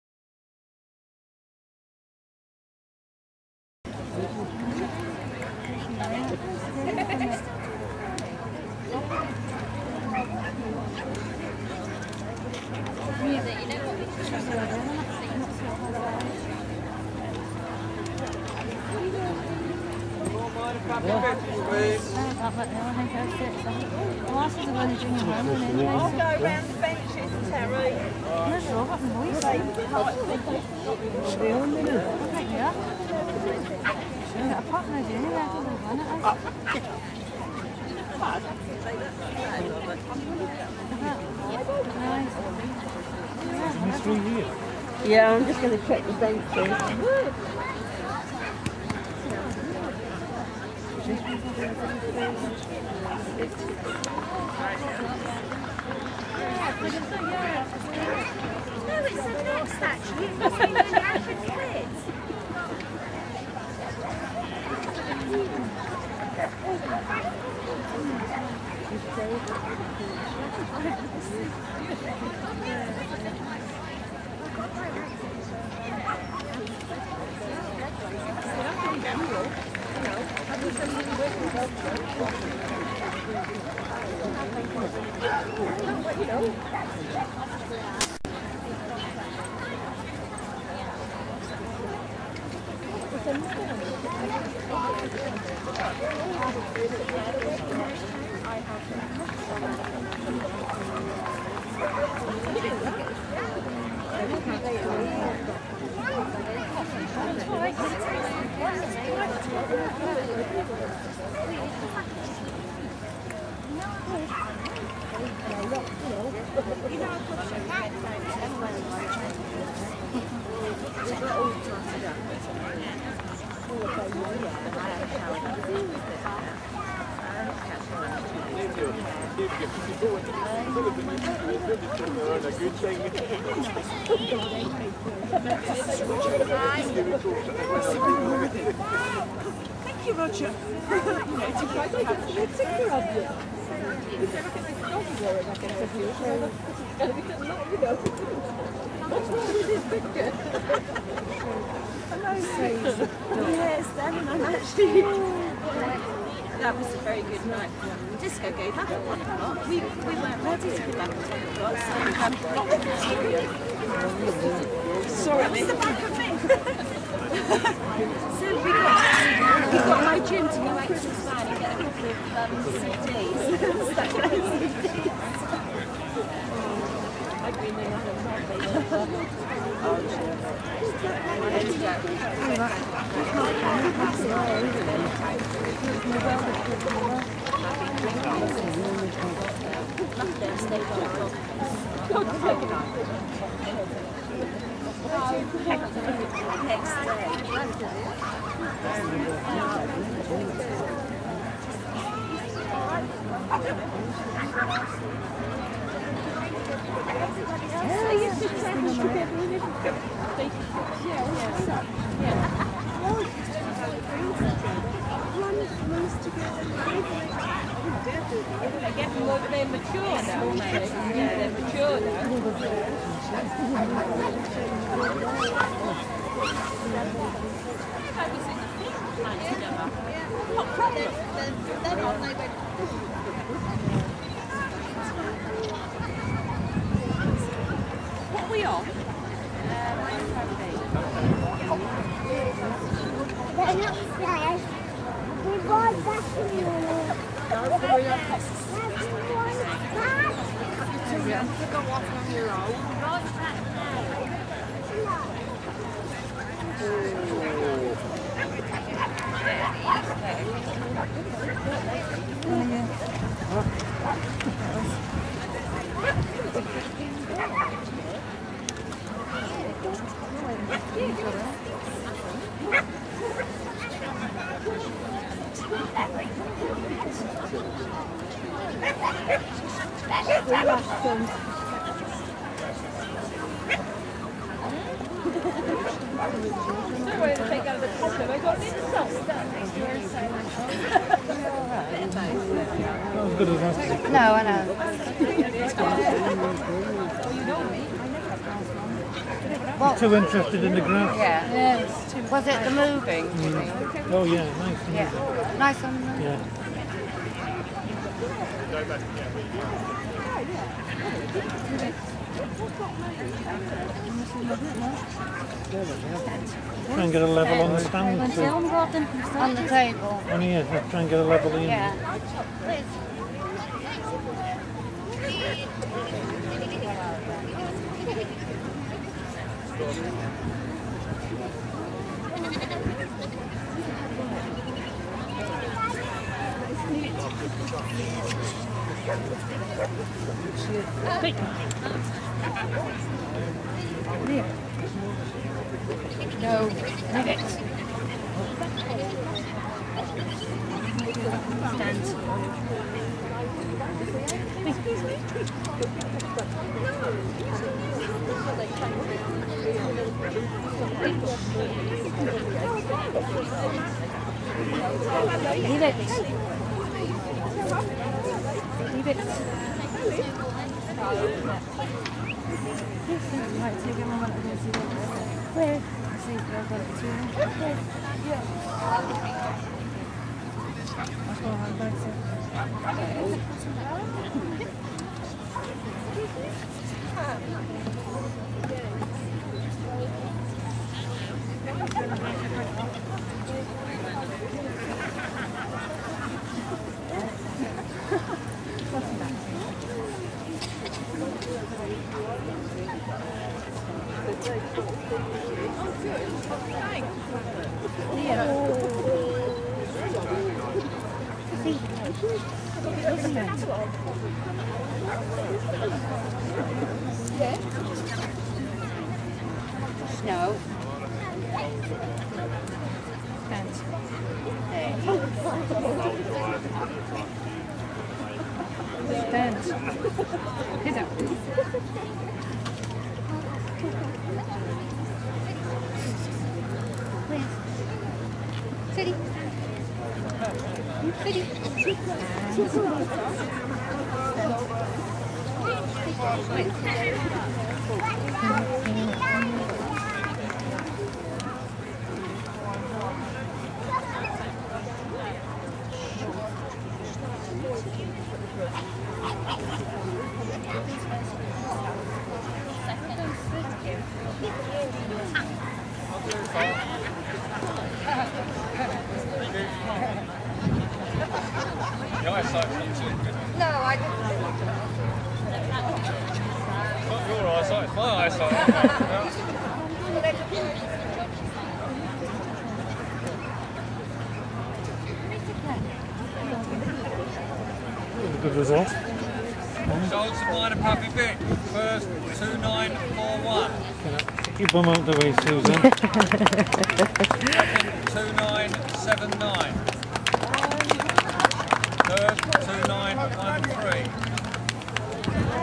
Wizz in Minor Puppy at WELKS 2007
Lemiz Gee Wizz it's Mispickel winning Minor Puppy at WELKS ch. Show 2007.
She then went on to win Best Puppy in Breed under Respected Judge Mrs Janet Knowles.